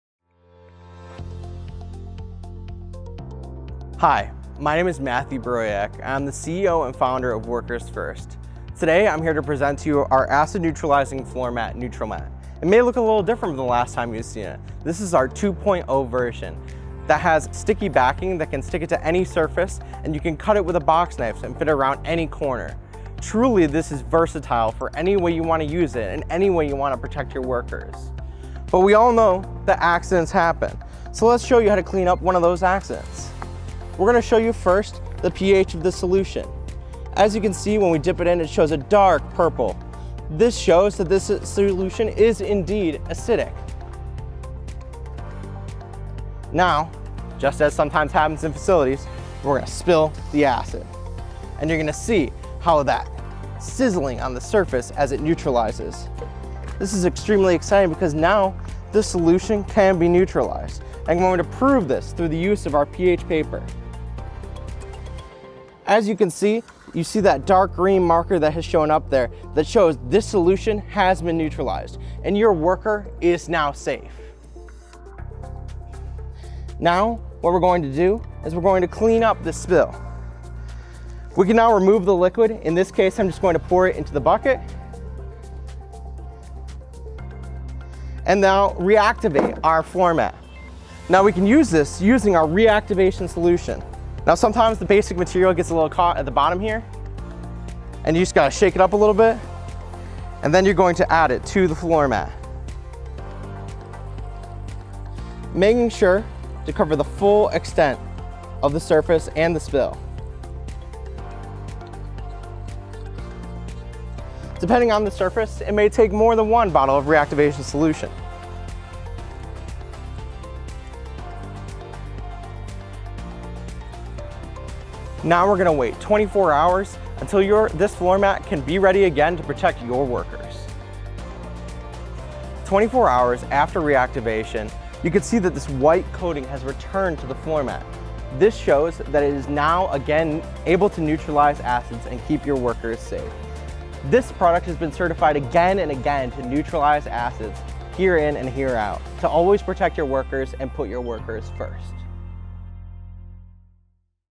Spill Bully Acid Neutralizing Mat Workers First
This floormat is designed for any space where acids are present. Ingrained in the surface of this mat is our patented acid neutralizing technology, which pre-emptively neutralizes acid molecules as soon as spills occur.